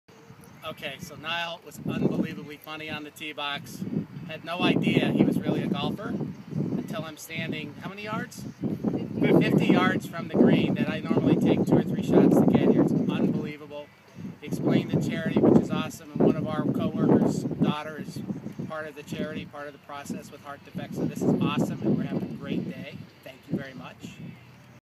Friends 4 Heart Charity Golf Fundraising Event
Nyle Pruitt hits long drives for the Friends 4 Heart Charity Golf classic helping with fundraising & awareness for children with congenital heart defects.